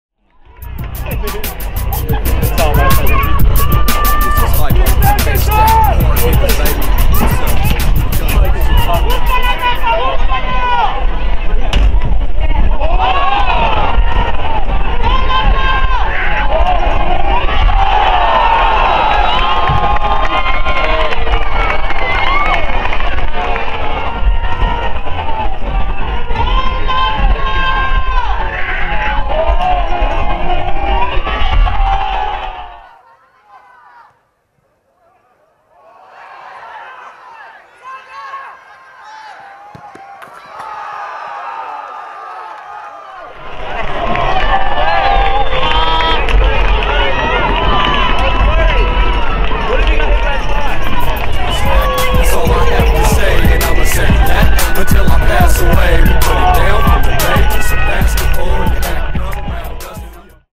East Bay FC Stompers vs. Napa Valley 1839 FC, 5/13/2017 (2)
Zano Mahic scores in the 77th minute off a 60-plus yard punt by goalkeeper Marcus Rojas that's flicked on for the assist by striker Greivin Pacheco to give the Stompers a late lead, providing the final score in a 2-1 road win against Napa Valley 1839 FC.

Pacheco heads the punt from Rojas ahead to Mahic, whose defender falls while trying to clear the ball. The burly midfielder eludes a second defender and slips the ball between the goalkeeper and the near post from the corner of the goal area.